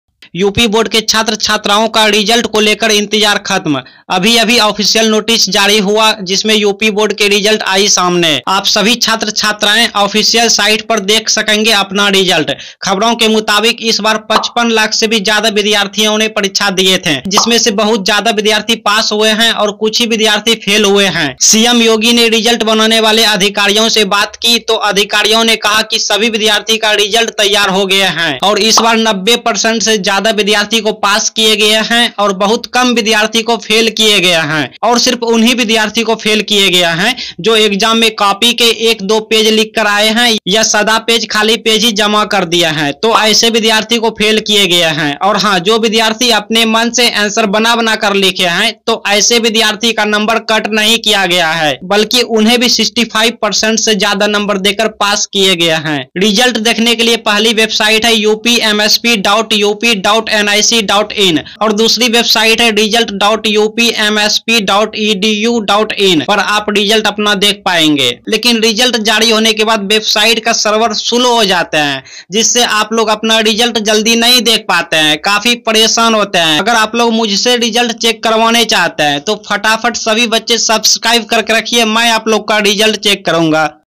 आज यूपी रिजल्ट सभी छात्र ऐसे करें चेक 🤗 | UP Board Result 2024 kab aayega
आज यूपी बोर्ड का रिजल्ट सभी छात्र ऐसे करें चेक 🤗 | UP Board Result 2024 kab aayega | UP Board Result 2024

About This Video :- iss video me bataye hain UP Board 2024 ka result kaise aaplog dekhenge up board ka result kaise kare check kaha se dekhen result iski puri jankari iss video me bataye gye hain

2024 यूपी बोर्ड का रिजल्ट आज जारी यहाँ से करें चेक
2024 यूपी बोर्ड का रिजल्ट कब घोषित होगा